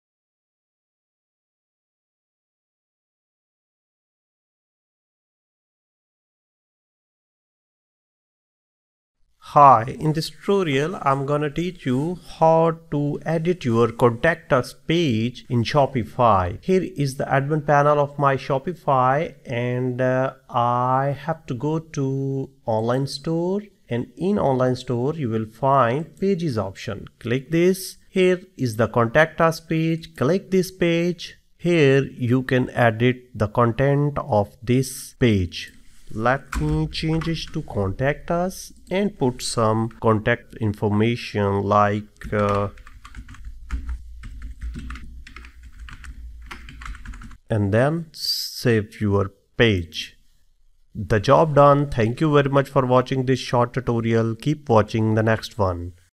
How to edit contact us page in shopify ll Edit shopify contact us page 2023
This is a common task that often needs to be done, and we'll walk you through the steps involved.

If you're looking for ways to improve your shopify store, then this video is for you!
In this video, we'll show you how to edit your contact us page easily and quickly, so that you can update it without hassle.If you're looking for tips on how to update your contact us page on shopify, then look no further!